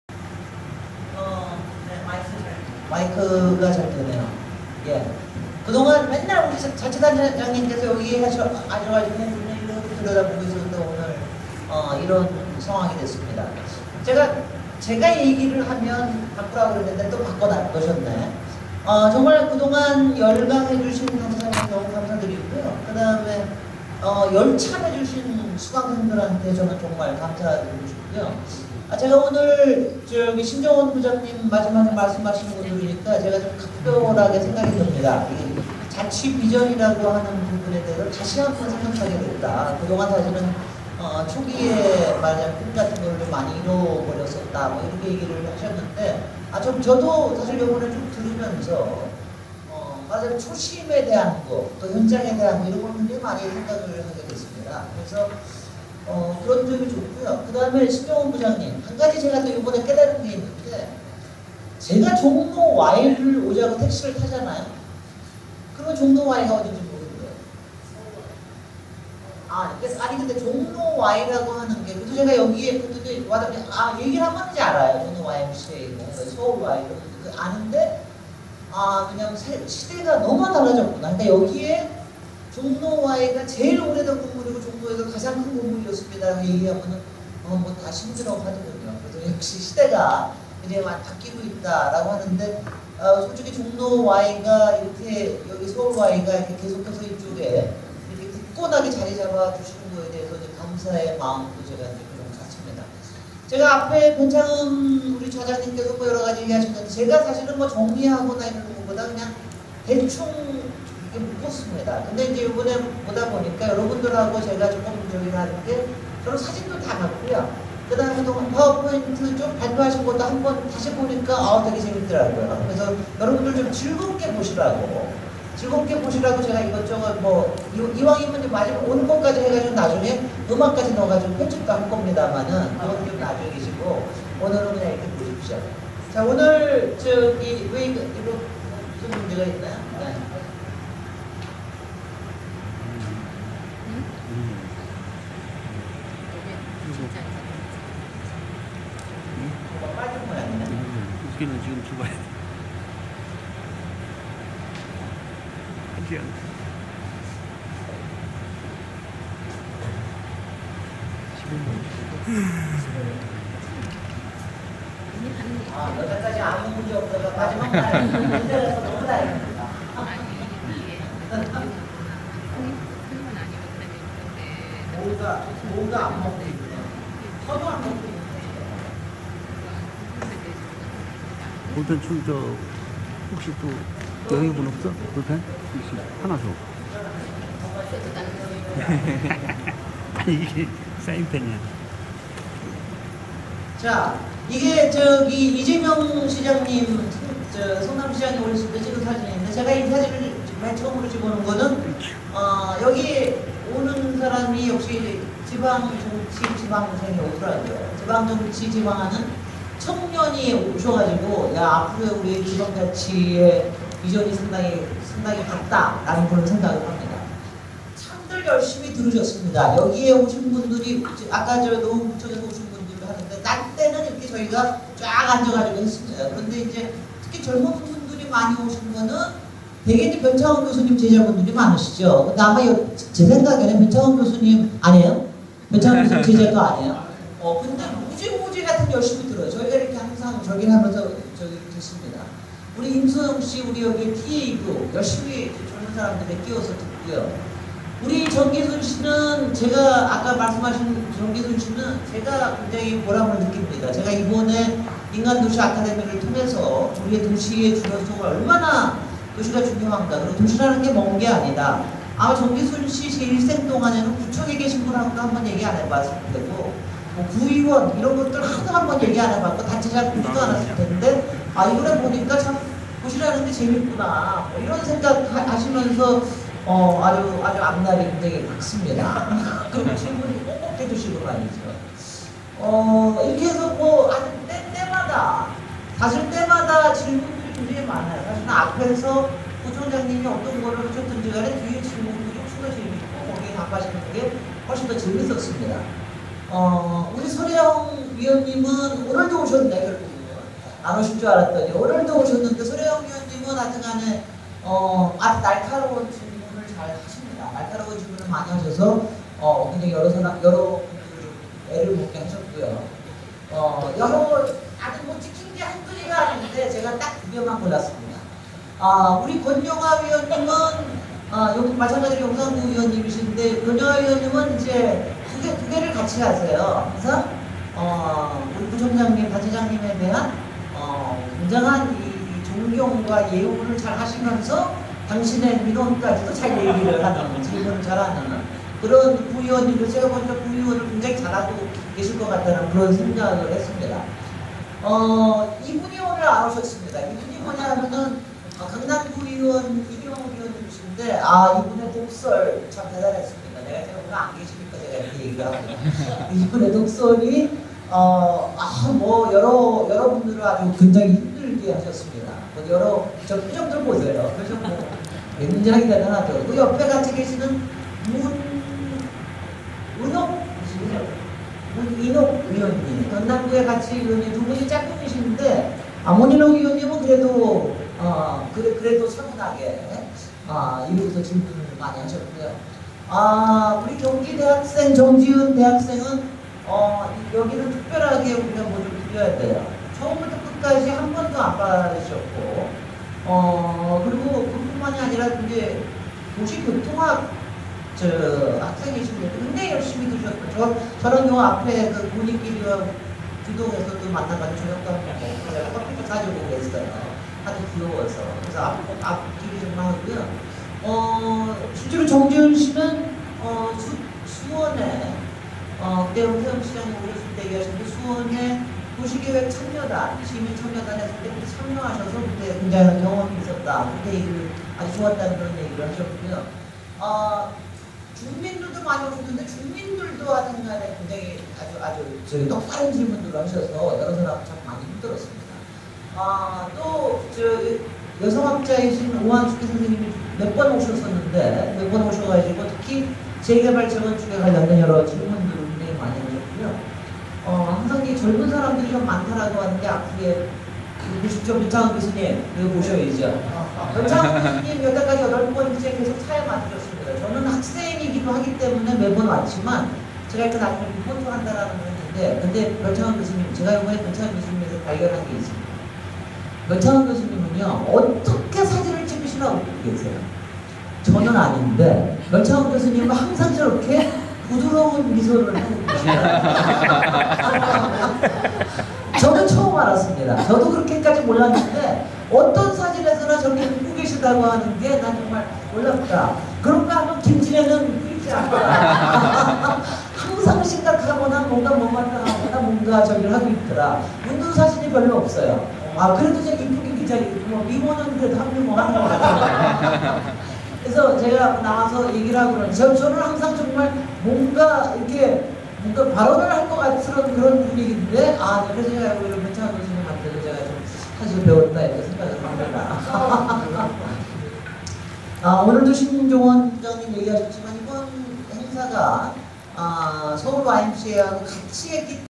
변창흠 교수 운영위원장 진행, 김진애 대표, 인간도시 아카데미 자치비전 실천강좌 종강소감, 토론 및 기념촬영 3
☞ 문화예술TV21 대표 송용운(문화나눔재단 이사장) 발행인 김옥연, 편집인 김영재
☞ 정기간행물등록번호 서울아-00666, 등록년월일:2008년9월23일